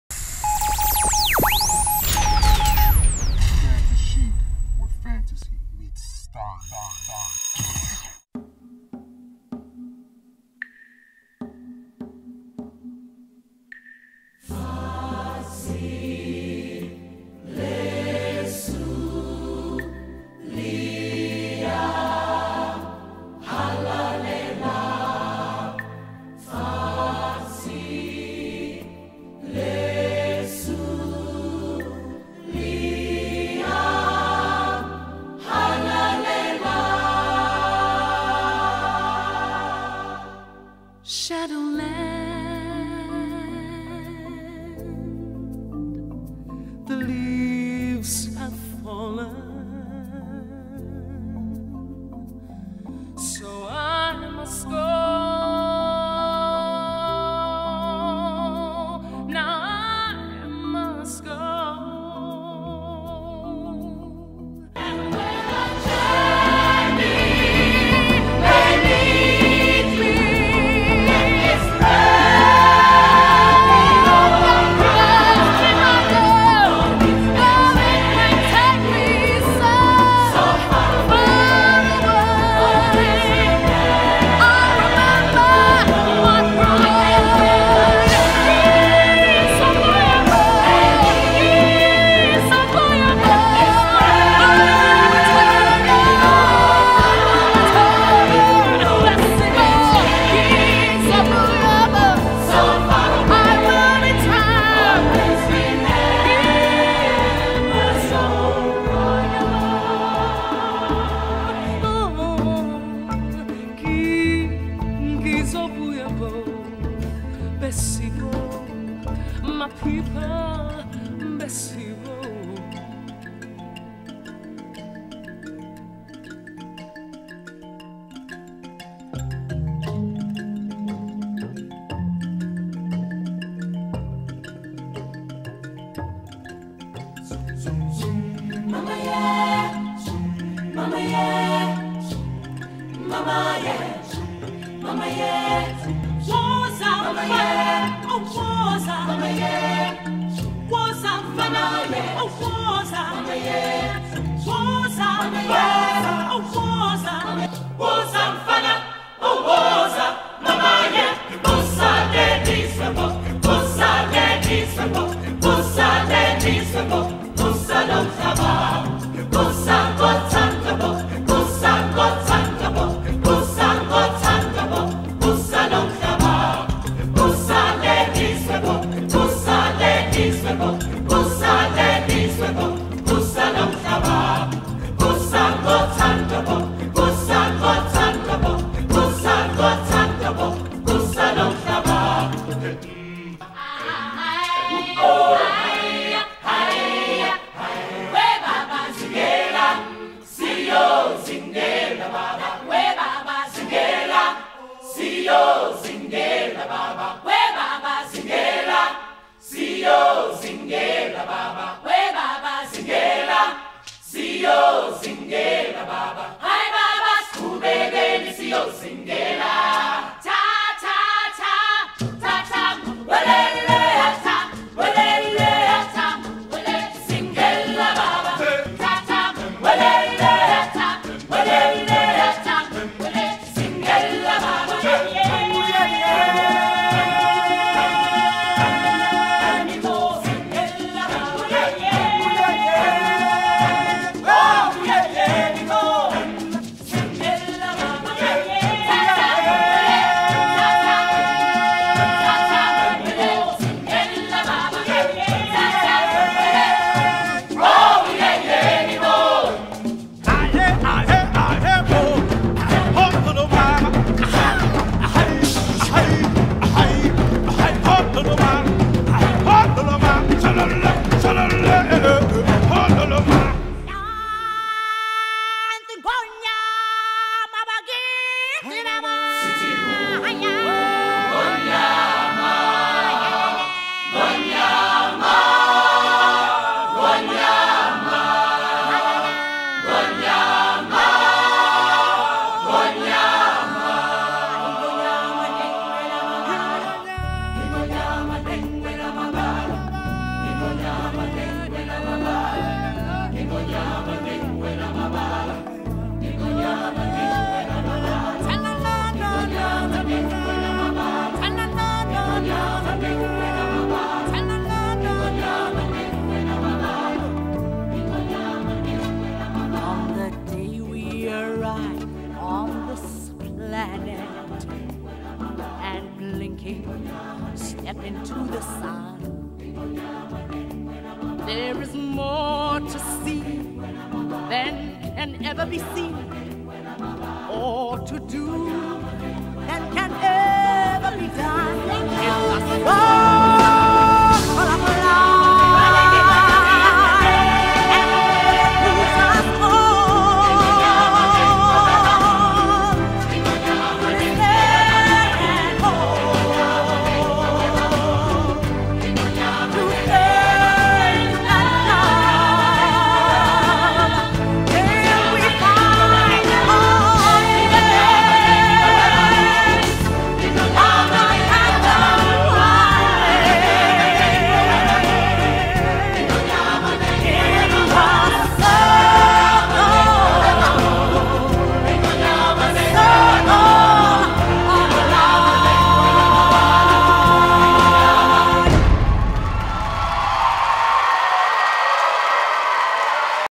ANIMAL INSTINCTS June 2014
Choreographed by Royce Reed, Lenora Spence and Clarence Jones. An modern piece inspired by The Lion King